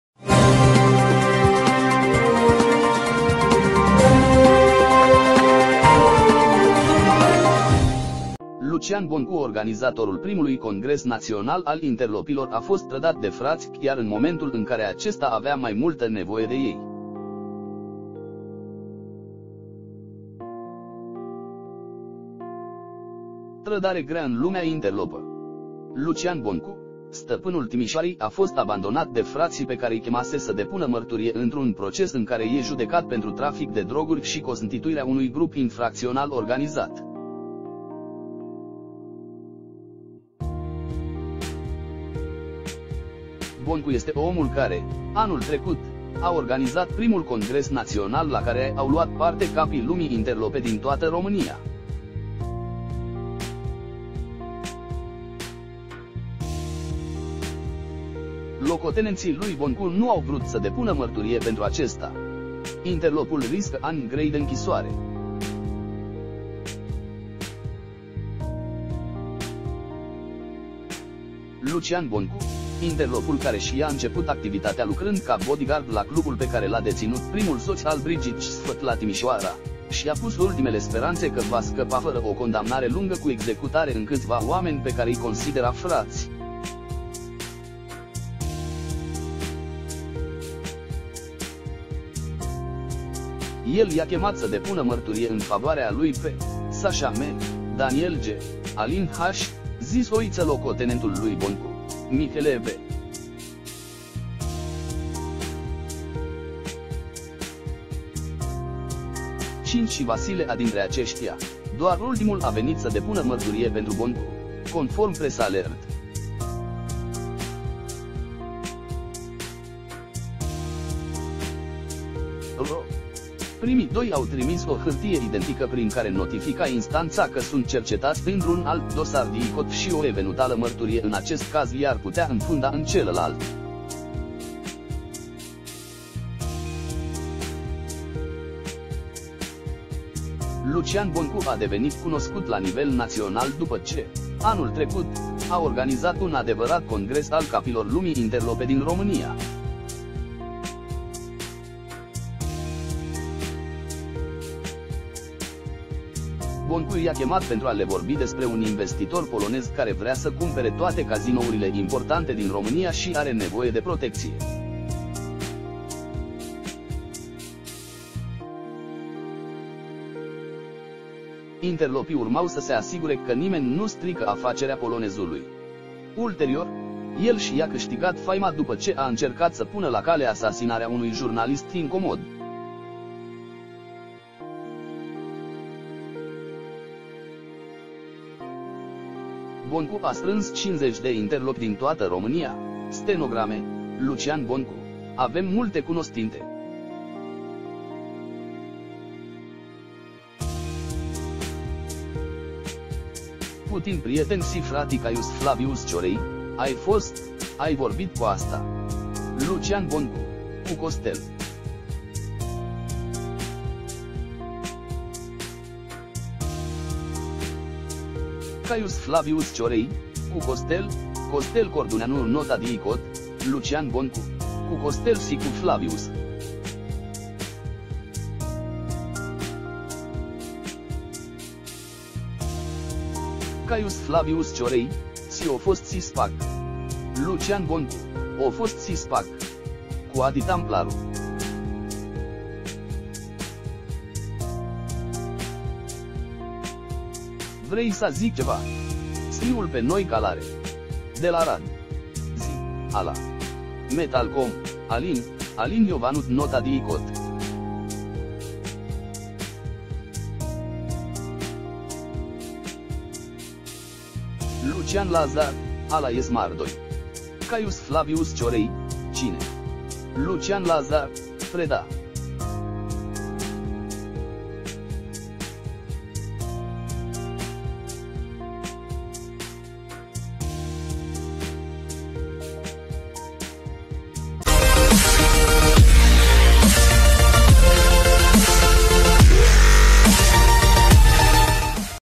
Trădare grea în lumea interlopă! Lucian Boncu, omul care a organizat primul congres al interlopilor
Lucian Boncu organizatorul primului ”congres național” al interlopilor a fost trădat de ”frați” chiar în momentul în care acesta avea mai multă nevoie de ei.
Trădare grea în lumea interlopă. Lucian Boncu, ”stăpânul Timișoarei” a fost abandonat de ”frații” pe care-i chemase să depună mărturie într-un proces în care e judecat pentru trafic de droguri și cosntituirea unui grup infracțional organizat. Boncu este omul care, anul trecut, a organizat primul congres național la care au luat parte capii lumii interlope din toată România.
Locotenenții lui Boncu nu au vrut să depună mărturie pentru acesta! Interlopul riscă ani grei de închisoare
Lucian Boncu, interlopul care și-a început activitatea lucrând ca bodyguard la clubul pe care l-a deținut primul soț al Brigittei Sfăt la Timișoara, și-a pus ultimele speranțe că va scăpa fără o condamnare lungă cu executare în câțiva oameni pe care-i considera frați. El i-a chemat să depună mărturie în favoarea lui pe: Sașa M., Daniel G., Alin H. (zis Oiță – locotenentul lui Boncu), Mihele B.V și Vasile A.. Dintre aceștia, doar ultimul a venit să depună mărturie pentru Boncu, conform pressalert.ro. Primii doi au trimis o hârtie identică prin care notifica instanța că sunt cercetați într-un alt dosar DIICOT și o evenutală mărturie în acest caz i-ar putea înfunda în celălalt.
Lucian Boncu a devenit cunoscut la nivel național după ce, anul trecut, a organizat un adevărat congres al capilor lumii interlope din România. Boncu i-a chemat pentru a le vorbi despre un investitor polonez care vrea să cumpere toate cazinourile importante din România și are nevoie de protecție. Interlopii urmau să se asigure că nimeni nu strică afacerea polonezului. Ulterior, el și-a câștigat faima după ce a încercat să pună la cale asasinarea unui jurnalist incomod.
Boncu a strâns 50 de interlopi din toată România (stenograme)
Lucian Boncu: Avem multe cunostinte... putini prieteni si frati...
Caius Flavius Ciorei: Ai fost, ai vorbit cu asta?
Lucian Boncu: Cu Costel?
Caius Flavius Ciorei: Cu Costel (Costel Corduneanu - nota DIICOT).
Lucian Boncu: Cu Costel si cu Flavius...
Caius Flavius Ciorei: Si o fost si Spak?
Lucian Boncu: O fost si Spak... cu Adi Tamplaru... Vrei sa zic ceva? SRI-ul pe noi calare! De la Arad... zii, ala... Metalcom, Alin (Alin Iovanut - nota DIICOT).
...Lucian Lazar: Ala e smardoi!
Caius Flavius Ciorei: Cine?
Lucian Lazar: Preda.